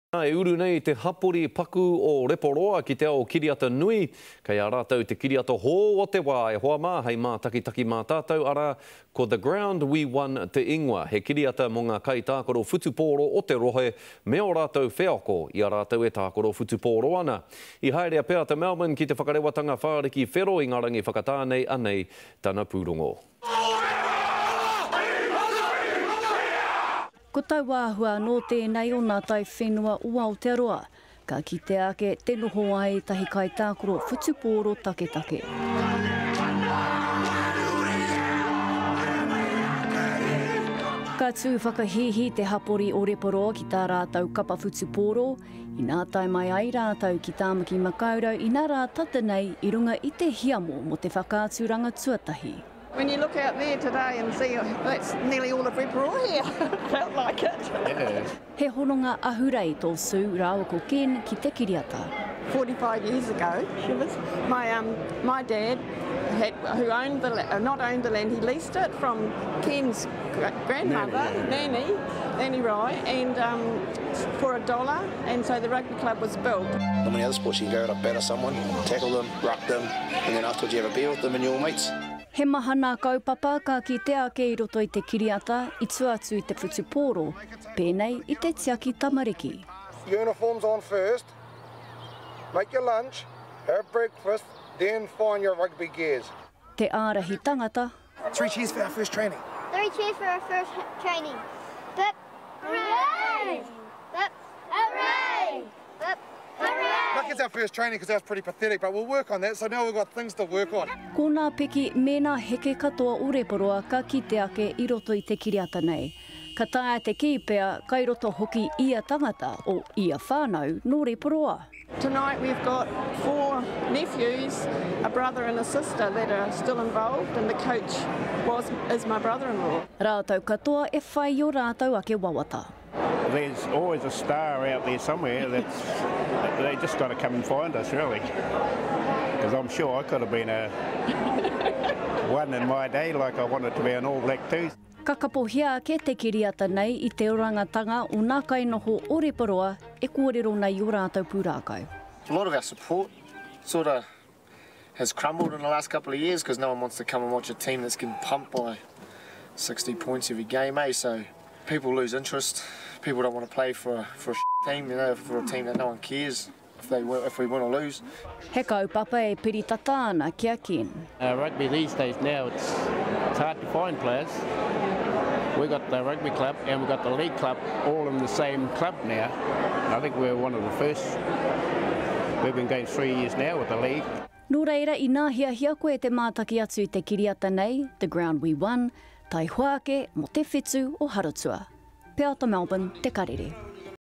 Reporoa rugby club hits the big screen
The small township of Reporoa is about to hit the big screen. The Ground We Won is the latest NZ film to come to the screens. It's a story about the challenges of manhood seen through the lives of rural rugby players in Reporoa.